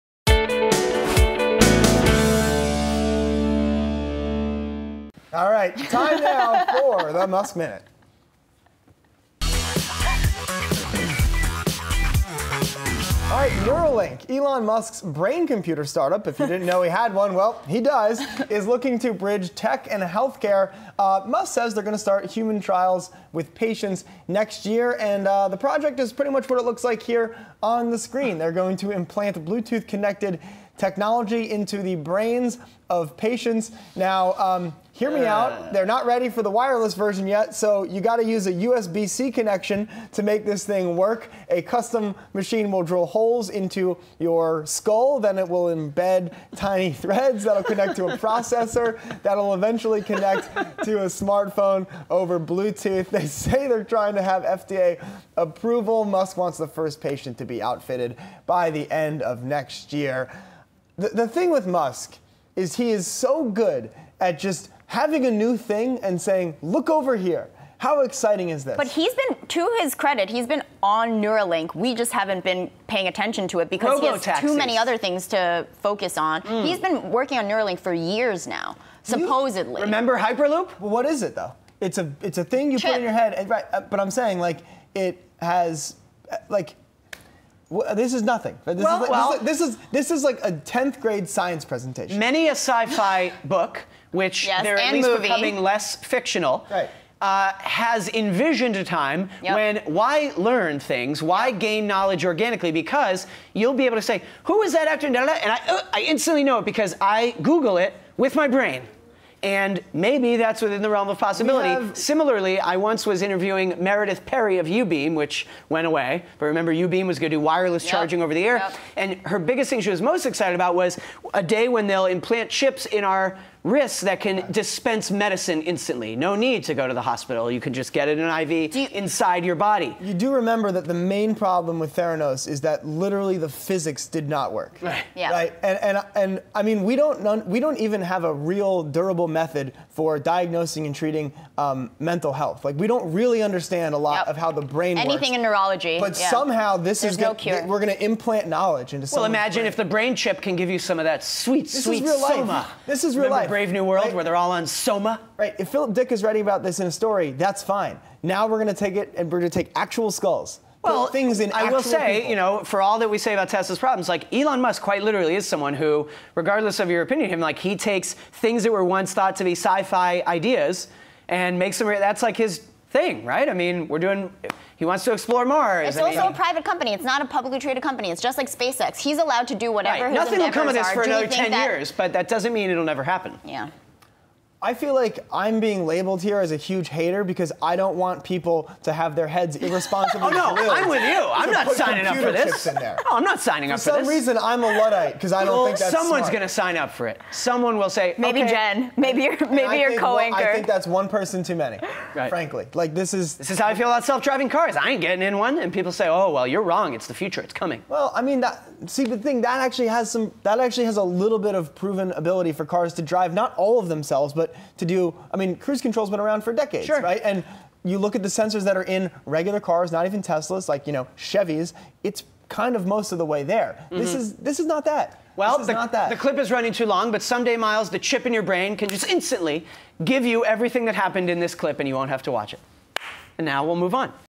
Elon Musk reveals plans for brain implant start up
Tesla CEO Elon Musk unveils plans for Neuralink brain implants.  Elon Musk's brain computer startup is looking to bridge tech and health care.  Musk hopes to plant bluetooth technology into the brains of patients.